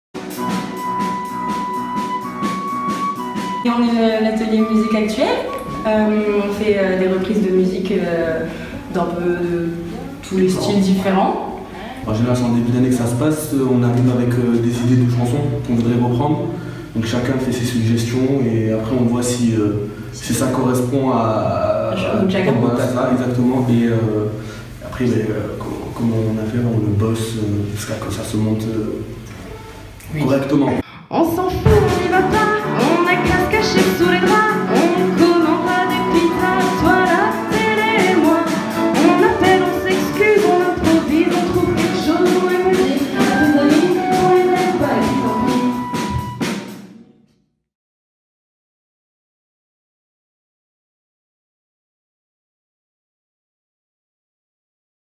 Julie Hosteins (Atelier de Musiques actuelles)
Présentation des activités de l'Ecole Municipale de Musique Danse et Théâtre Dominique Boudot : Julie Hosteins (Atelier de Musiques actuelles)